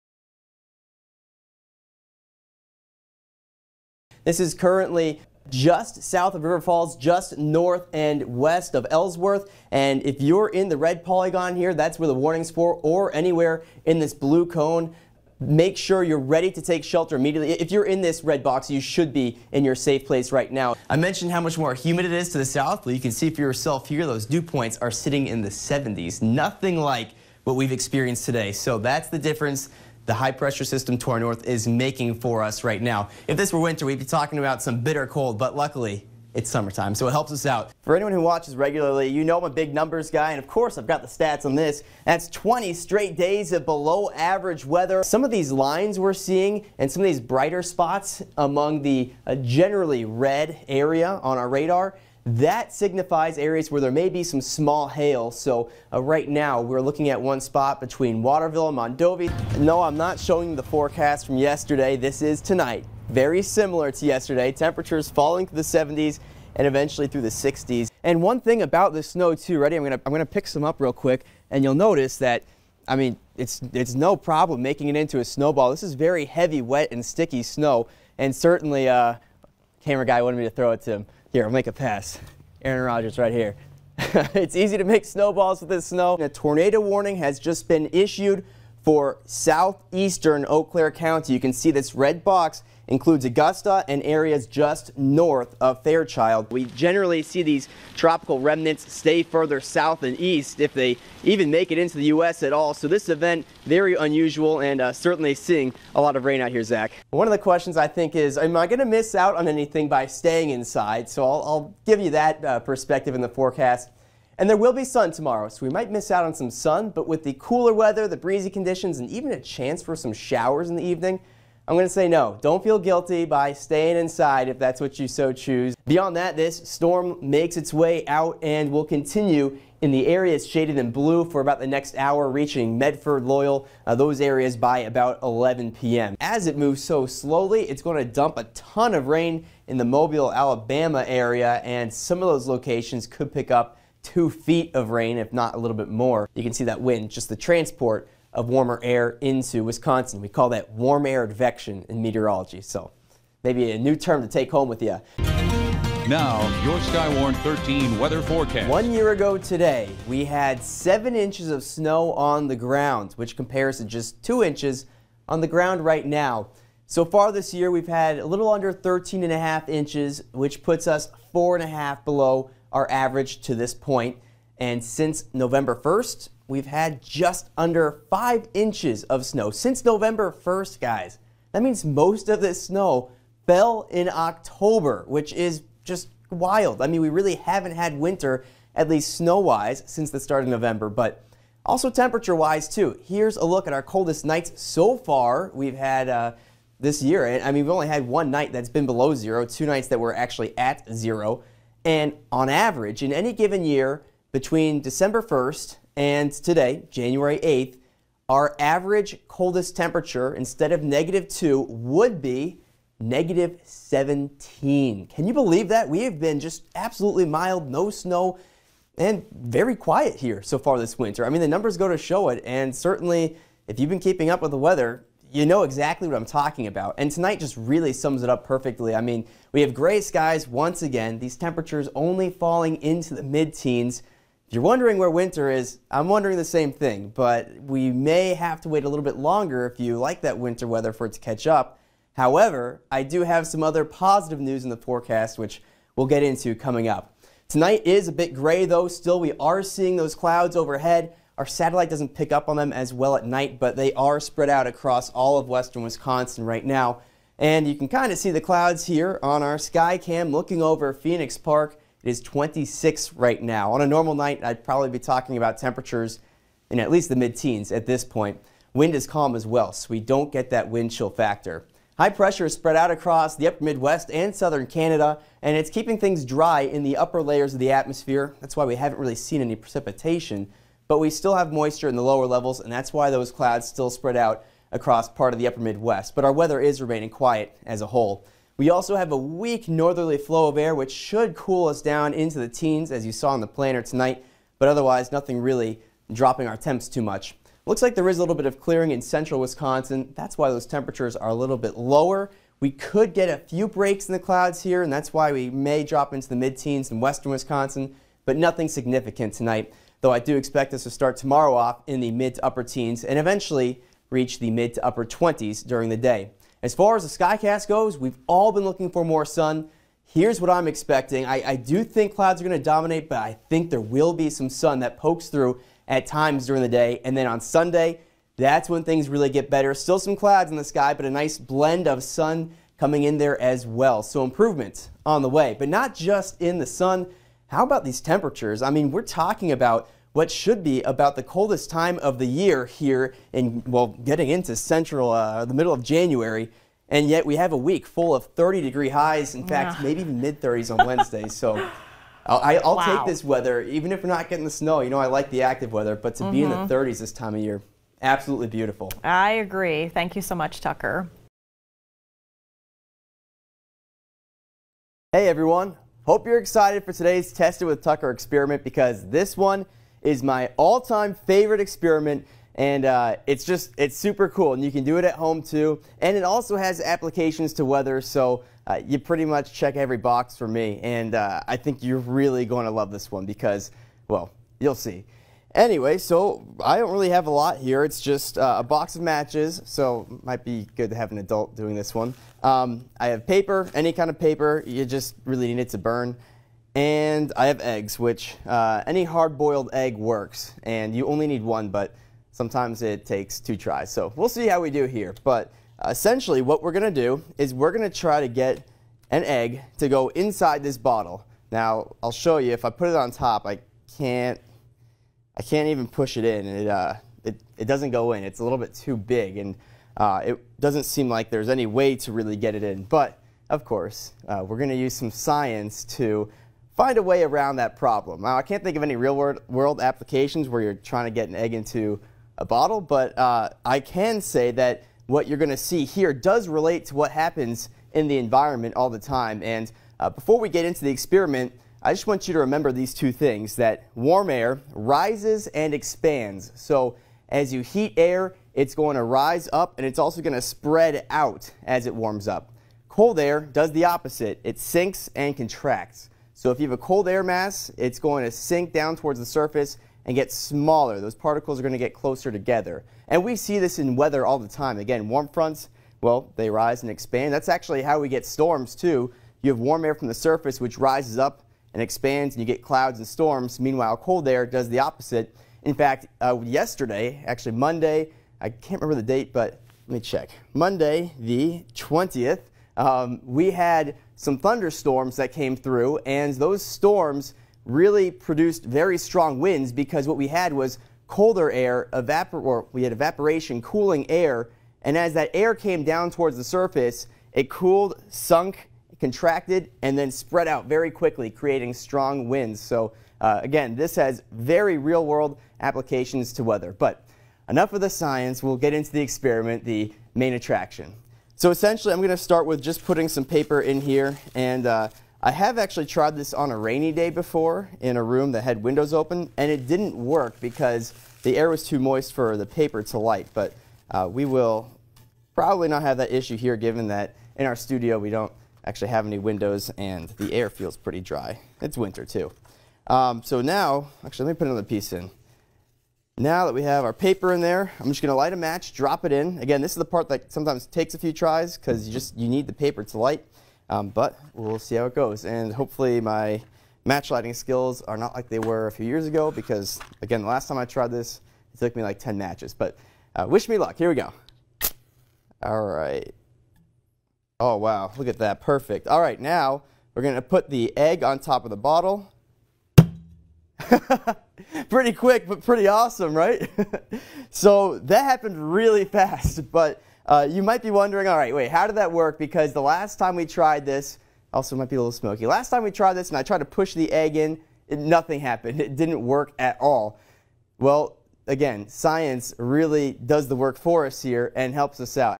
Tucker Antico Weather Resume Reel (2021)
I am currently a broadcast meteorologist at WEAU-TV in Eau Claire, WI.
I hold a BS in meteorology from Penn State.

Montage: 0:04
Full Weather: 2:36
Test It With Tucker: 6:04